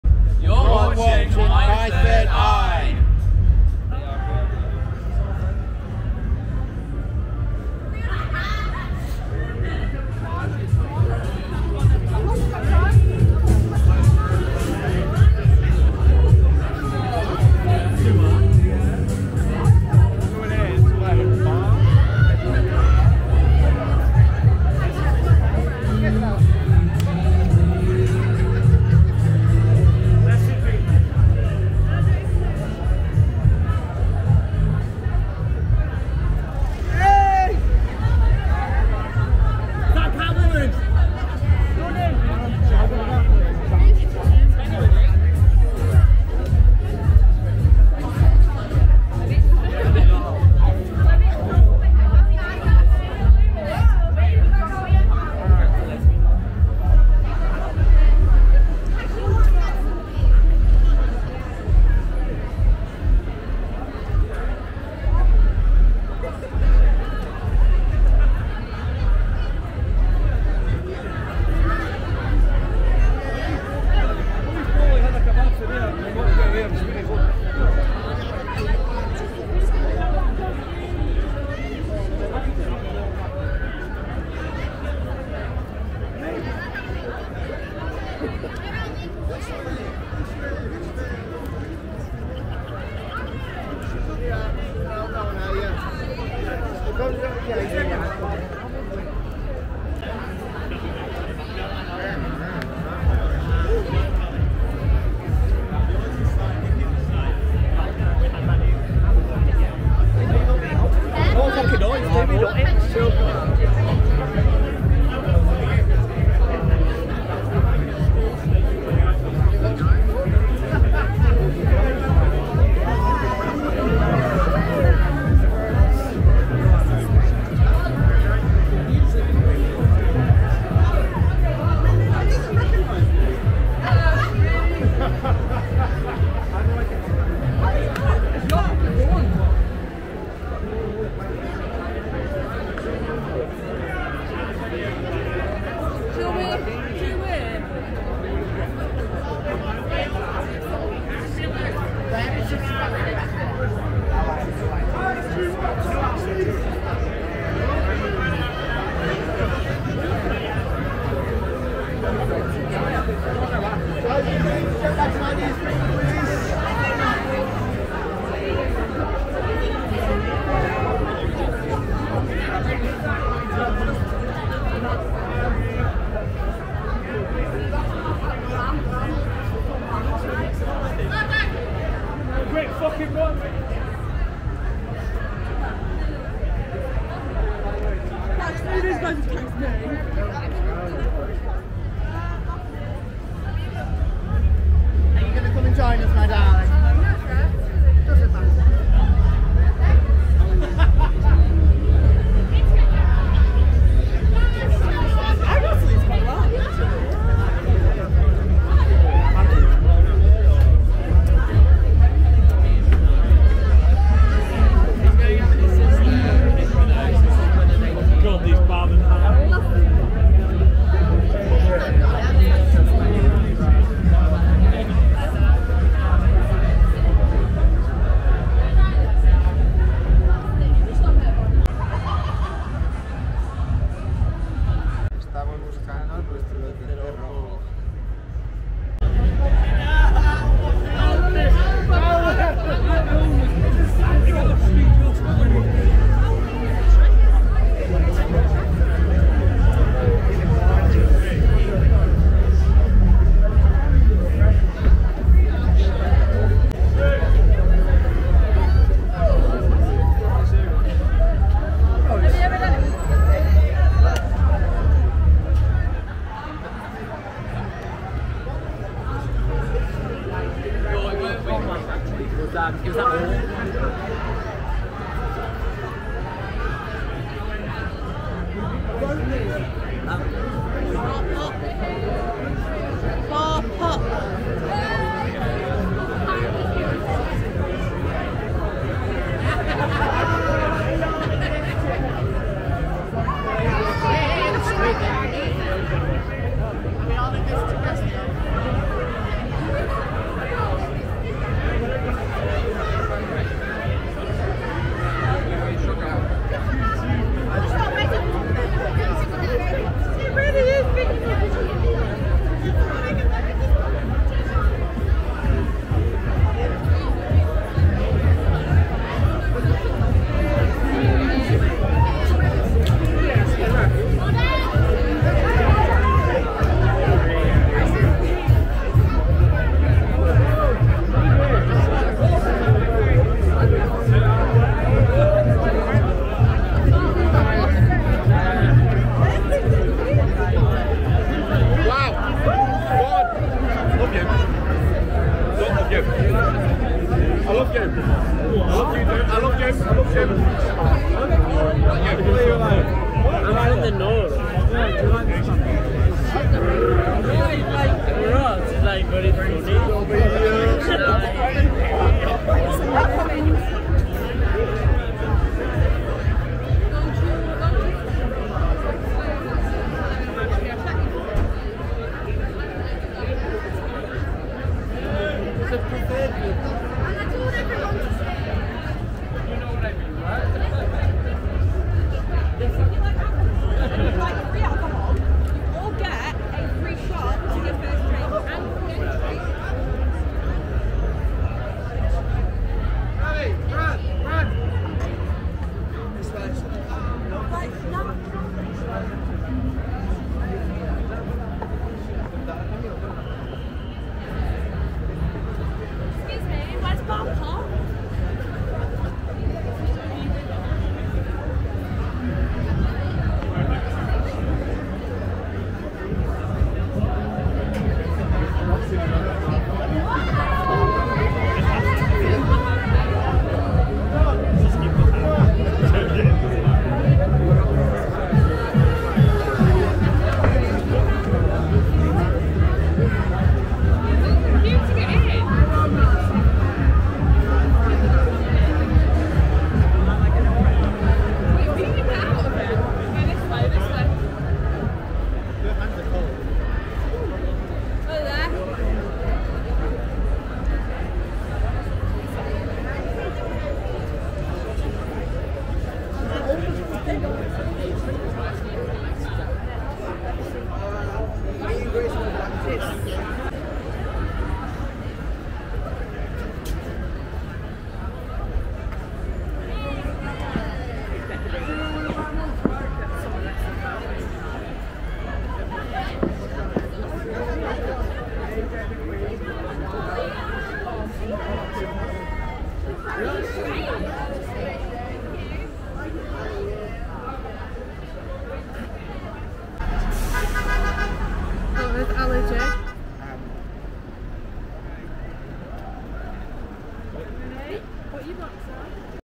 Manchester Halloween Night Out | Gay Village | 4k
Walking through Manchester's Gay Village / Canal Street on the cities Halloween Night celebration weekend. In this video, we walk through the bar and club area of the gay village and look at the sights and scenes of the Halloween night celebration 2022. Filmed in 4k/60fps.